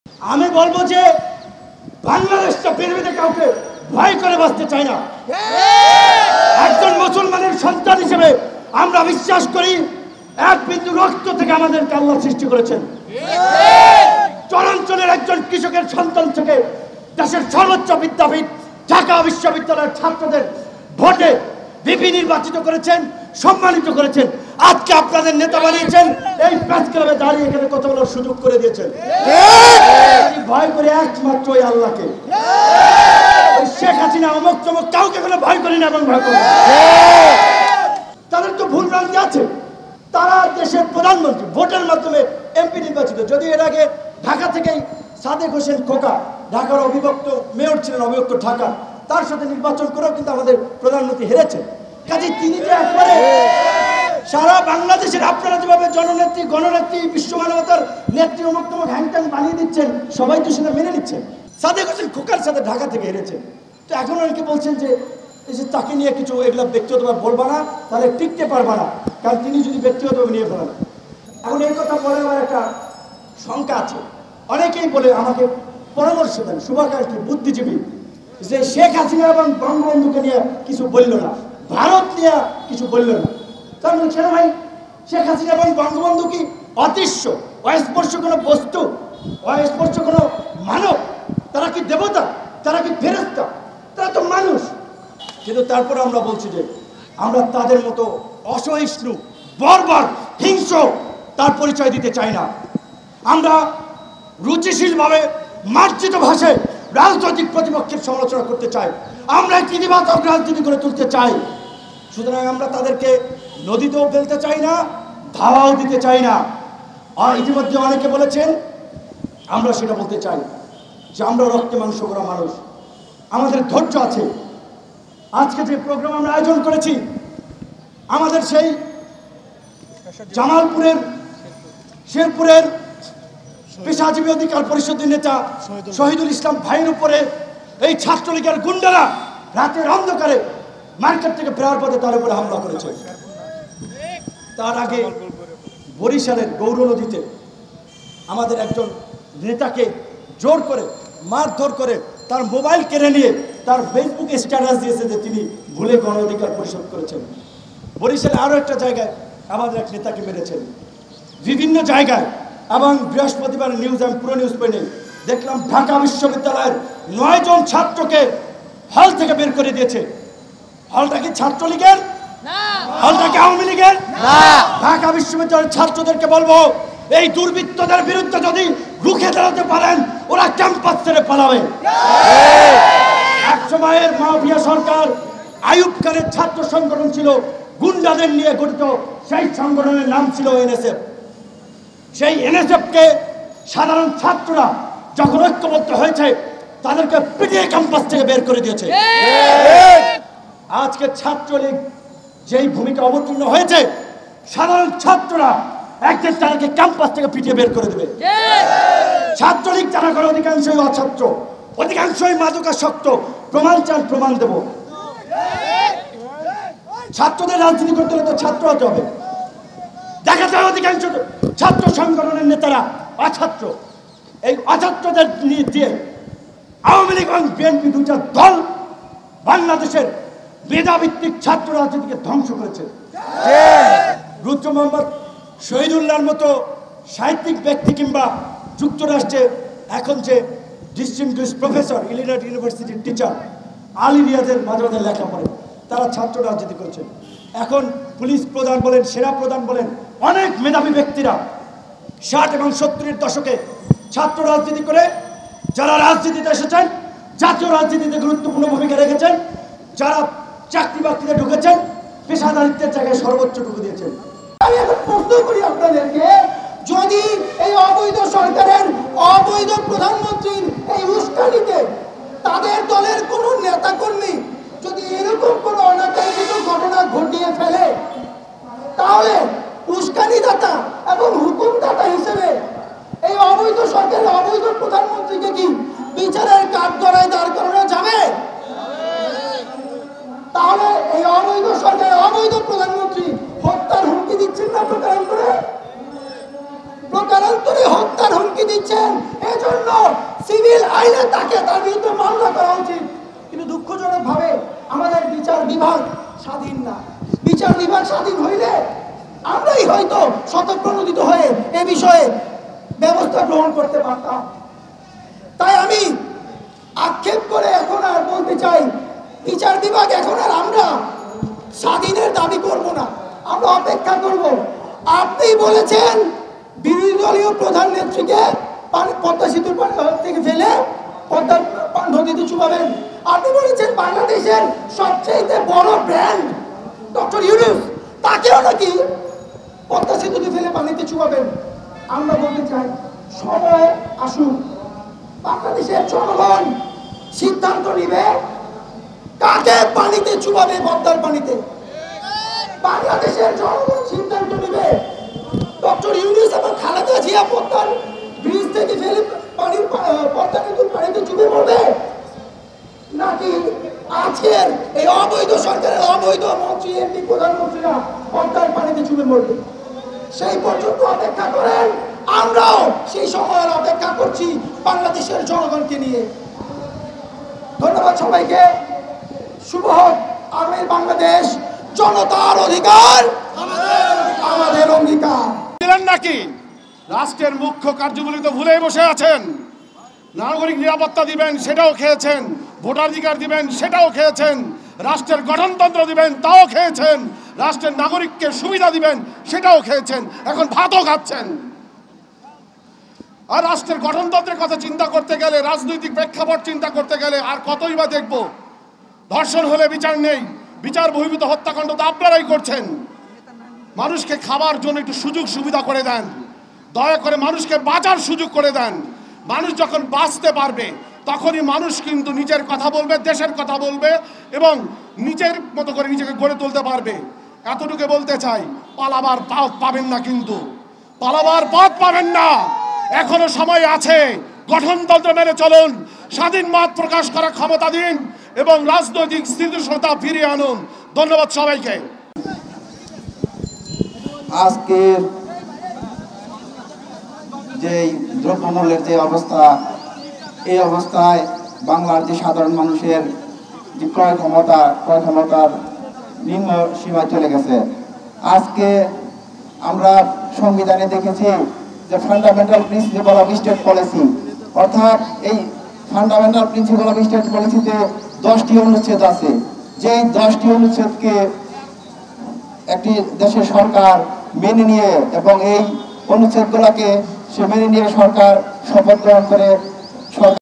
মাথা নত করবো না !!! আল্লাহ ছাড়া কারো কাছে , বললেন ভিপি নূর
সাদেক হোসেন খোকার কাছে হেরেছেন !!! স্মরণ করিয়ে দিলেন ভিপি নূর